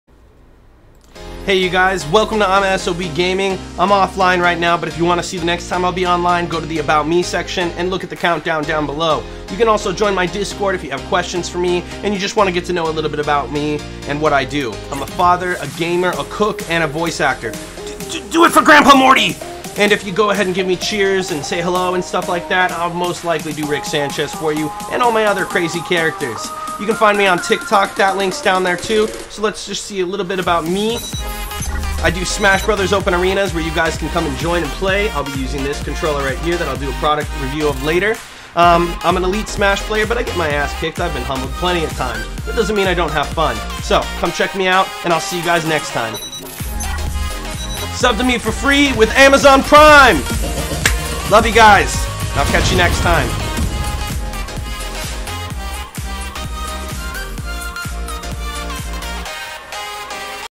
immasob TTV trailer!
Hey everyone I live stream on twitch and I want you to get to know a little about me in just 60 seconds!

Got a minute to spare?? Great!

You can follow me and find all the links to everything else here on youtube or straight from my twitch!

"Do it for grandpa Morty!"